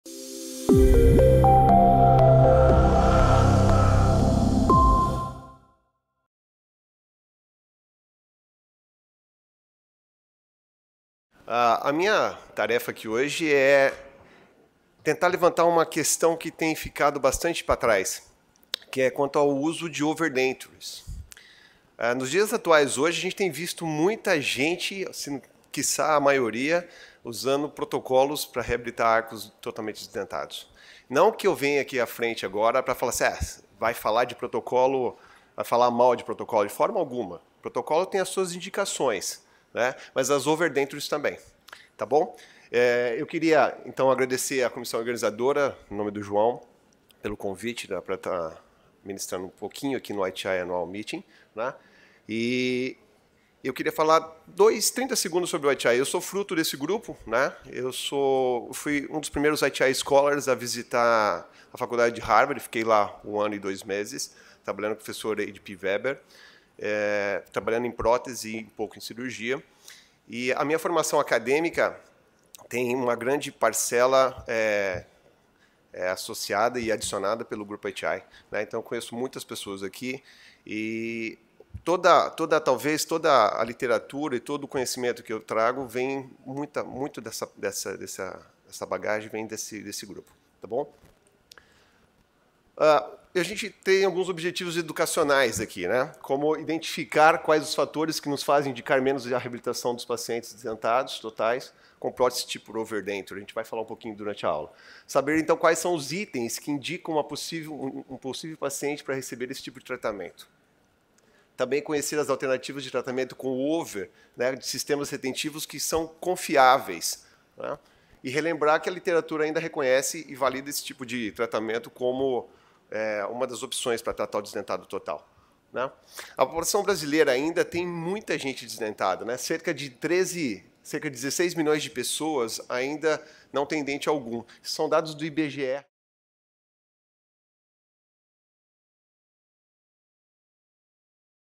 Overdentures – Por Que Nos Esquecemos Delas? | Fabio Sanches Magalhaes Tunes
ITI Academy Congress Lecture Preview: Overdentures – Por Que Nos Esquecemos Delas? by Fabio Sanches Magalhaes Tunes.